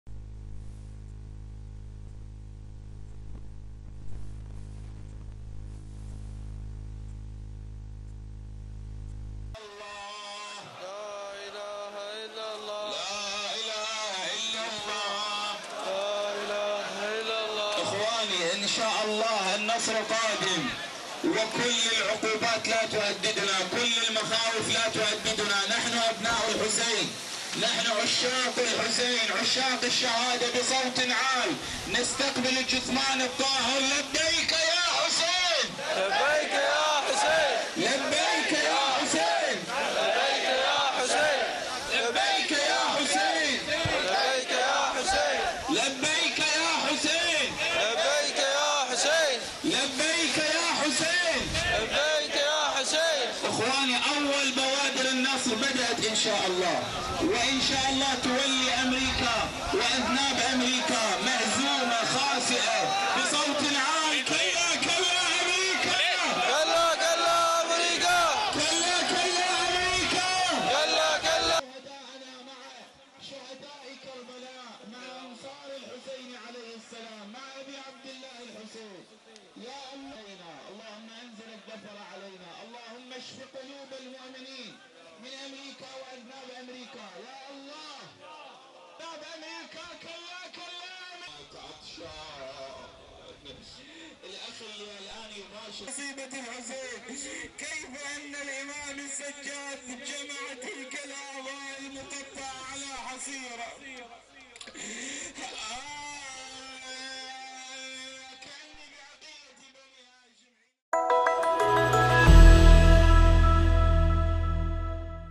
El Mühendis'in Basra'daki Cenaze Törenine Binlerce Kişi Katıldı
İranlı General Kasım Süleymani’yle birlikte Cuma günü ABD tarafından düzenlenen operasyonda ölen Haşdi Şabi Komutan Yardımcısı Mehdi El Mühendis için bugün Irak’ın Basra kentinde geniş katılımlı bir cenaze töreni düzenlendi.